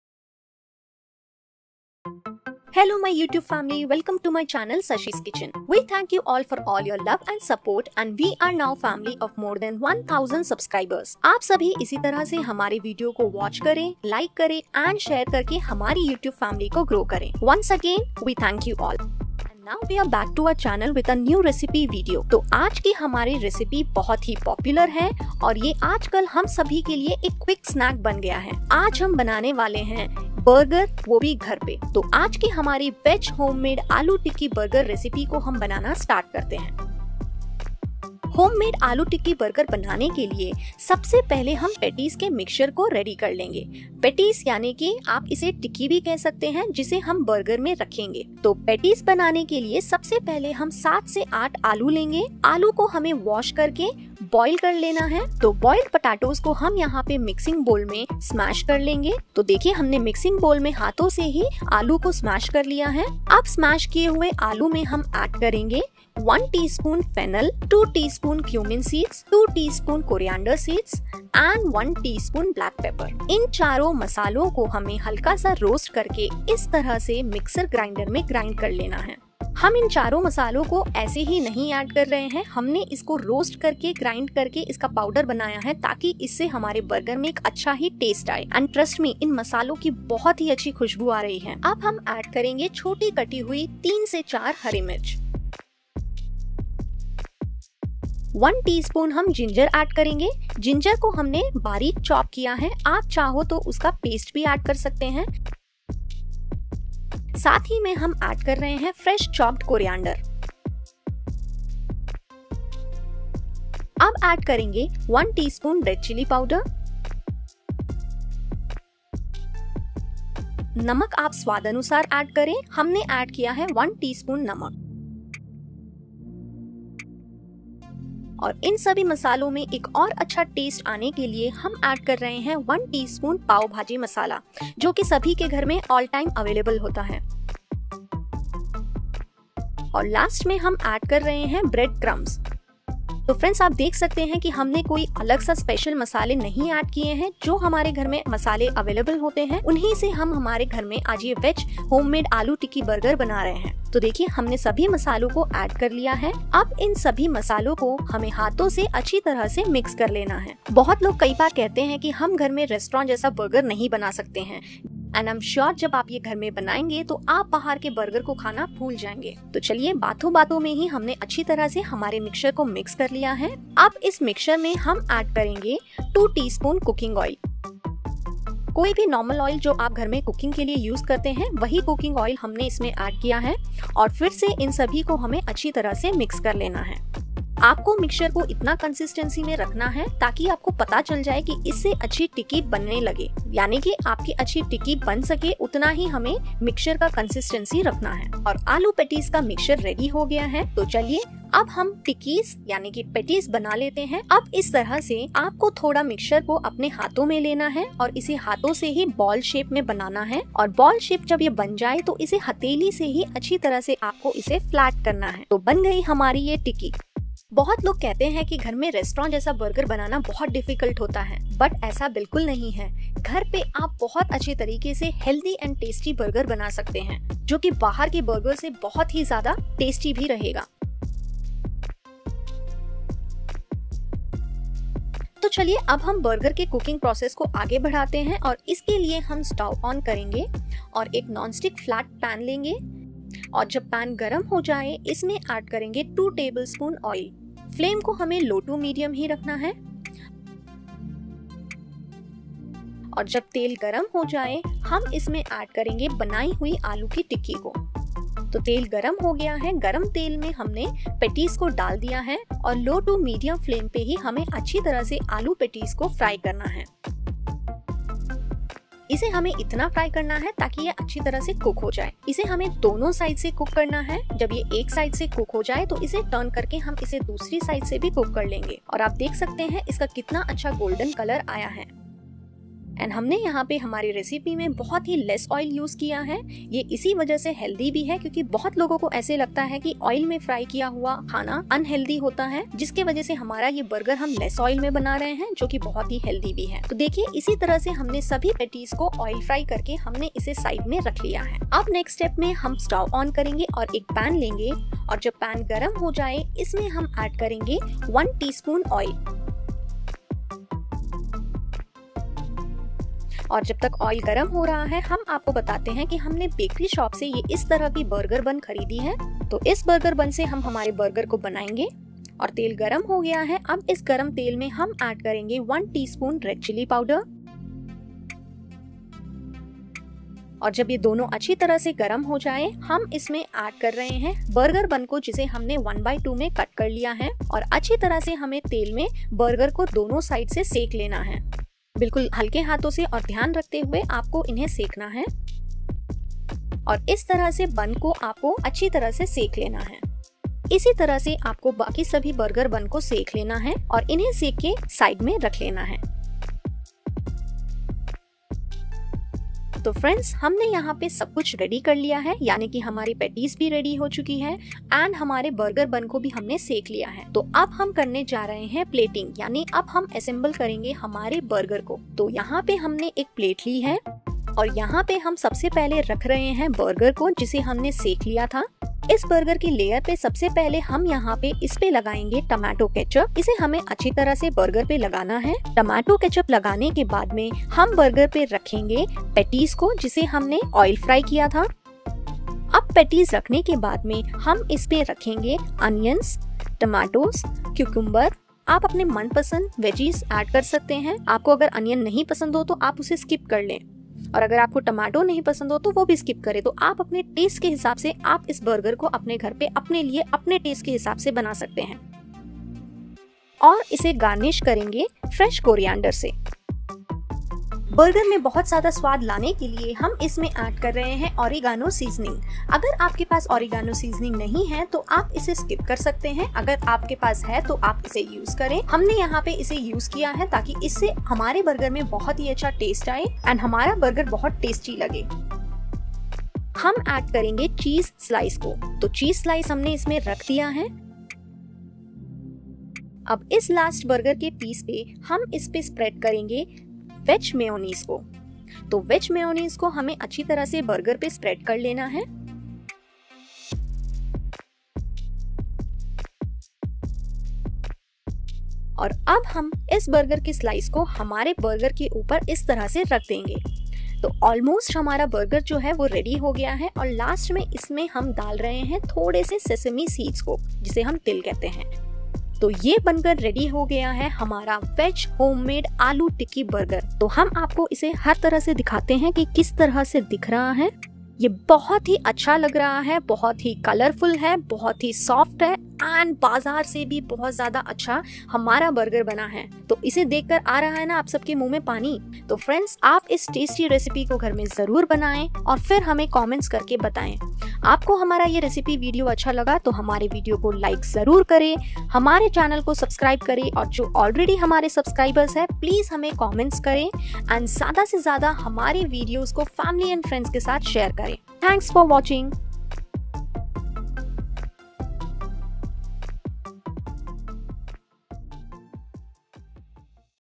aloo tikki burger recipe 🍔| Easy & Delicious| Homemade Veg Burger | Shashi's Kitchen
Hello Friends,
Welcome To Shashi's Kitchen YouTube Channel

We Have Just Added New Cooking Recipe Video. Hope You All Will Try.
How To Make Aloo Tikki Burger | Easy & Delicious| Homemade Veg Burger At Home?

🔸 INGREDIENTS 🔸
4 Burger Bun
3 Chopped Green Chilies
1Tsp Fennel Seeds
1Tsp Black Pepper
2 Tsp Coriander Seeds
2 Tsp Cumin Seeds
Fresh Chopped Coriander
1 Tsp Chopped Ginger
1Tsp Pav Bhaji Masala
Sesame Seeds
Oil/Butter For Cooking
7-8 Boiled Potatoes
Bread Crumbs
Cheese Slice
Veg Mayonnaise
Seasoning Oregano (Optional)
Onion
Tomatoes
Cucumber

If You Like Our Videos Please Give Us